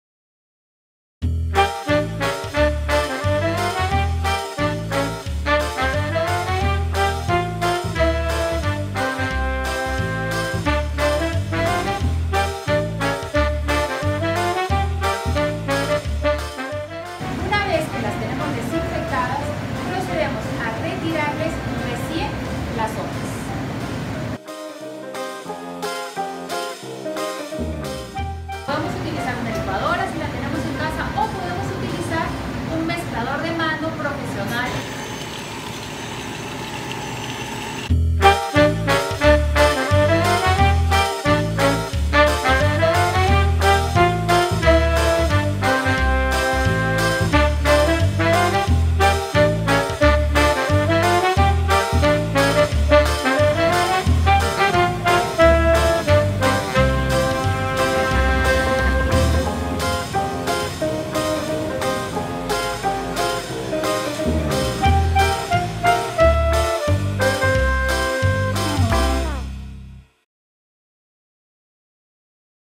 Aprende a preparar un sorbetto de fresa
Prepara un helado en solo 5 pasos, con la receta de la maestra heladera de la Cafeladería 4D, Ana María Bugosen.

Además, conoce la ruta del helado en Lima en el blog